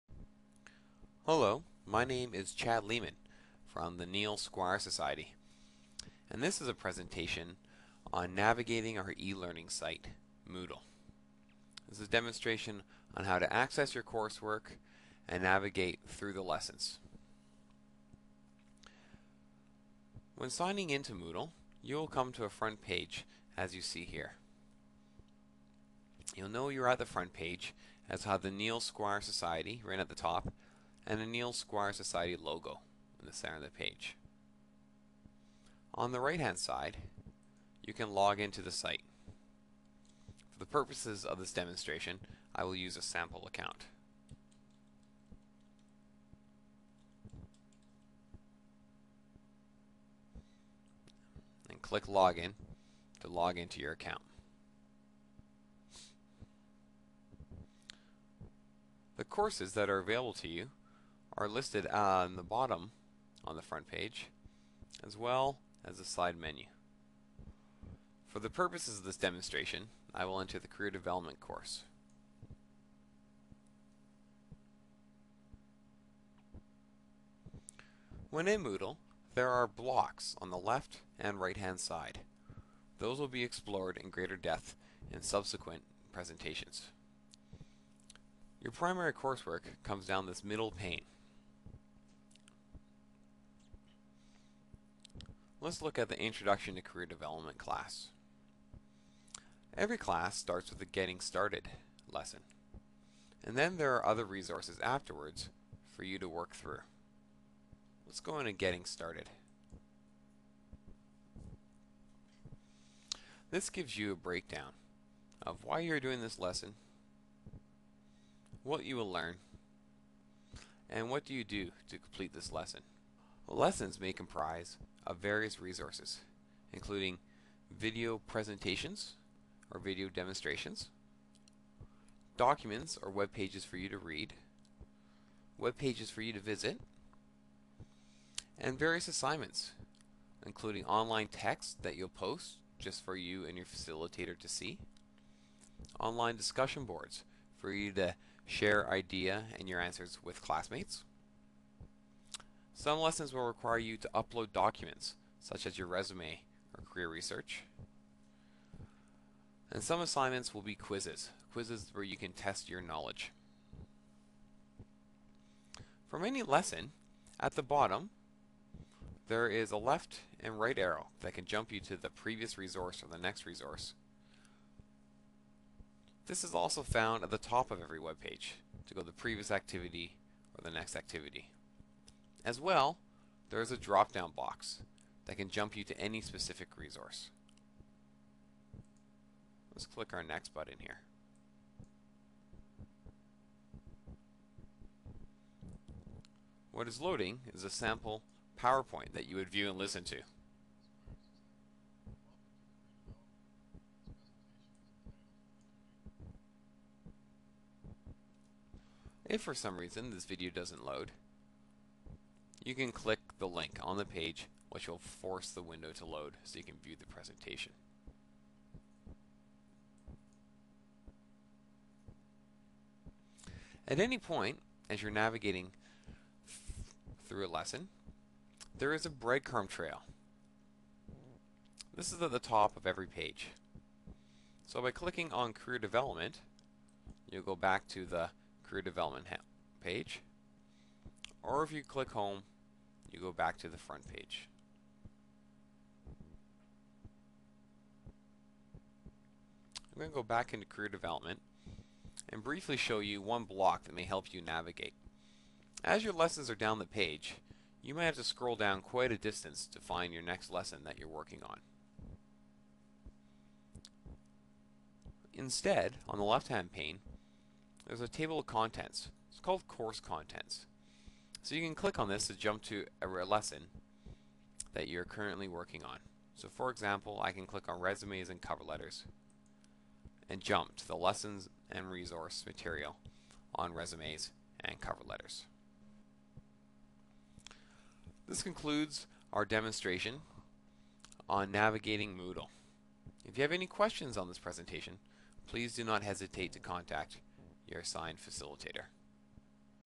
moodle Navigation
How to Navigate around the Moodle page created by the Neil Squire Society